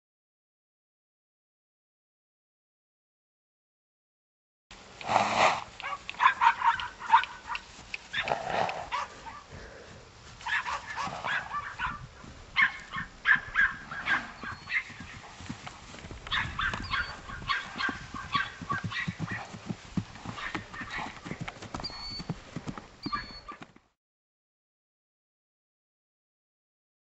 Welsh Pony Filly for sale
2 year old Section B Welsh Filly for sale. Stands 13 hands and will mature to 13:1 to 13:2.